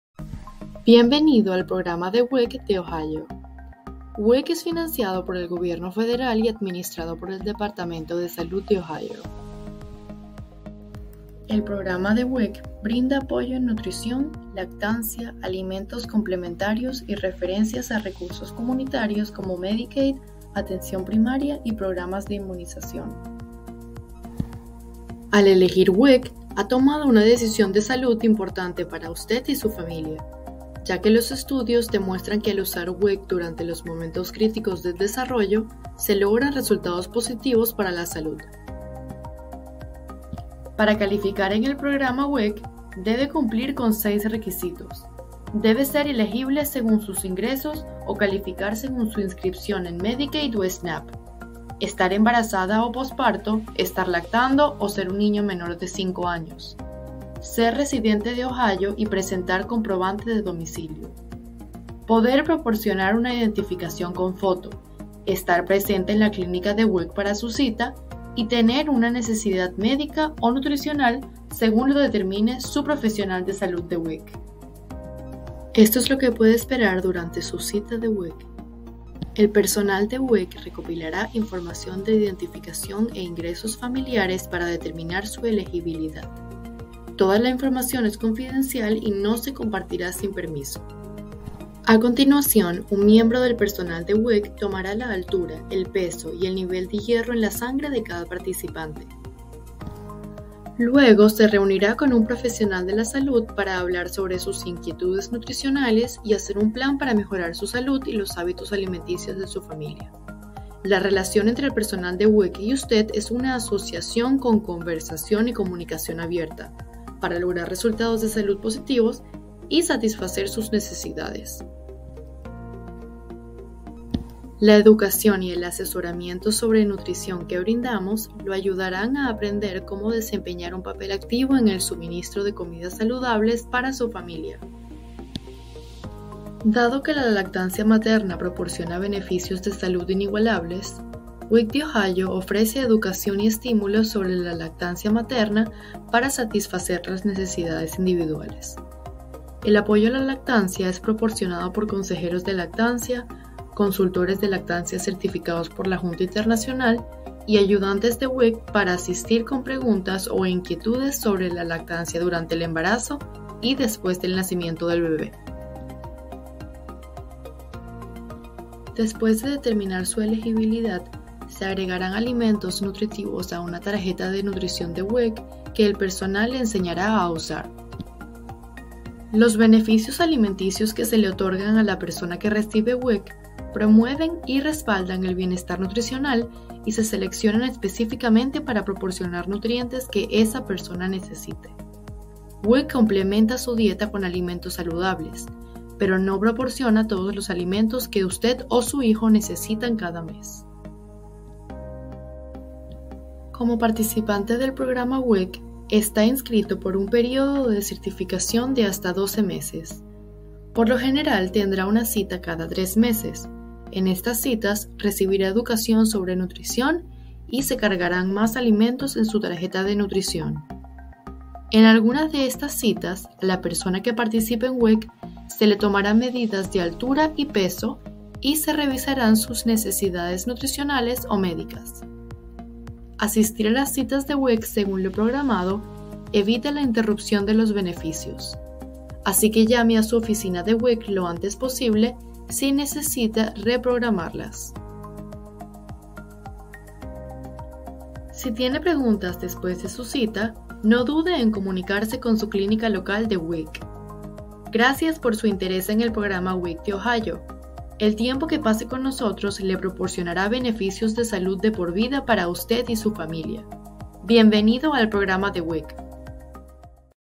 Bienvenido a WIC
Una breve descripción del programa WIC y cómo el programa lo beneficiará a usted y a su hijo.